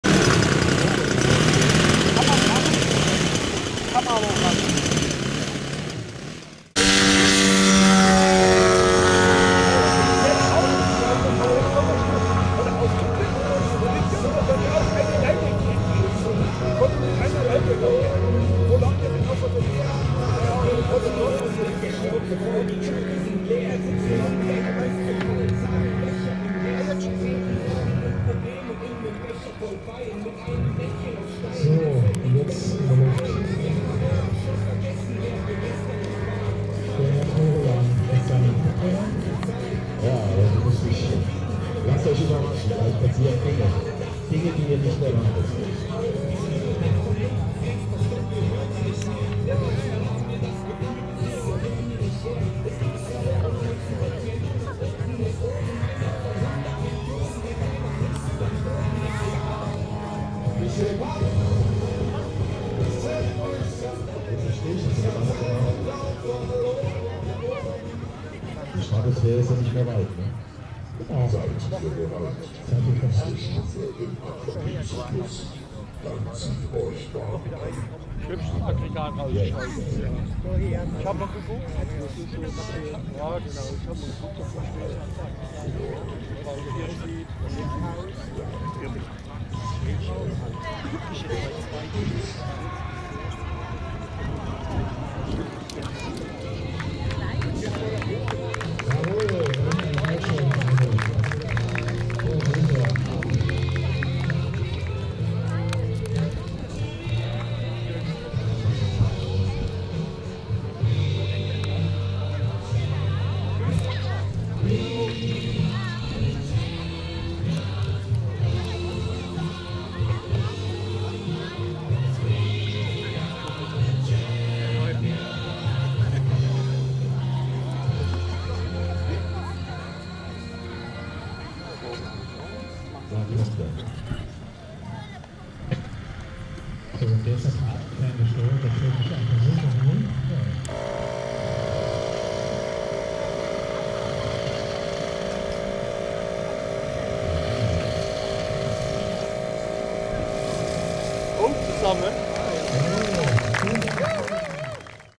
RC PARACHUTE SKYDIVE FREEFALL FALLSCHIRMSPRUNG (Modellflugschau Wehrheim 2024)
RC Fallschirmsprung mit kurzem Freifall

Geflogen bei der Modellflugschau der Fliegergruppe Hochtaunus e. V. in Wehrheim, am 29./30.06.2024.